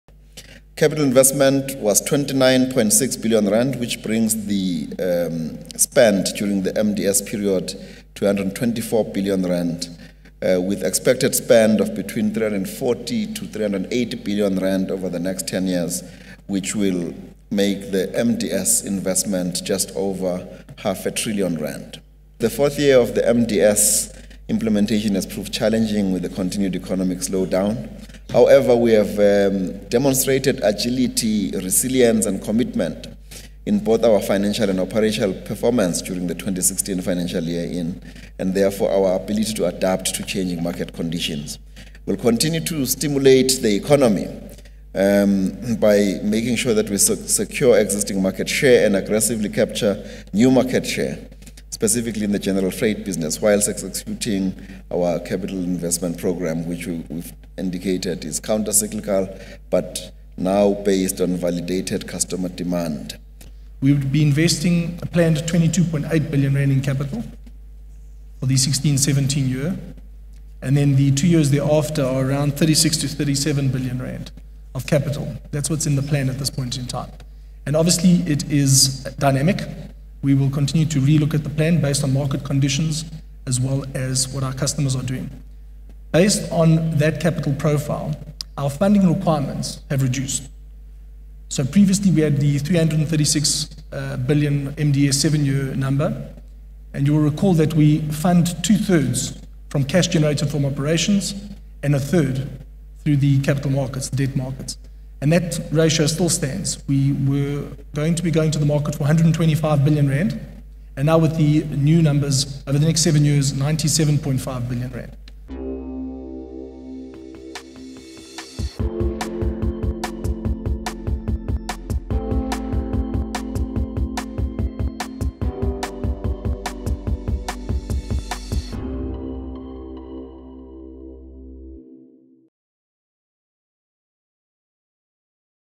Transnet pares back capex as commodity slump impacts demand
State-owned freight logistics group Transnet has pared back its 2016/17 capital expenditure (capex) plan in line with its strategy of “validating” demand ahead of moving ahead with major investments.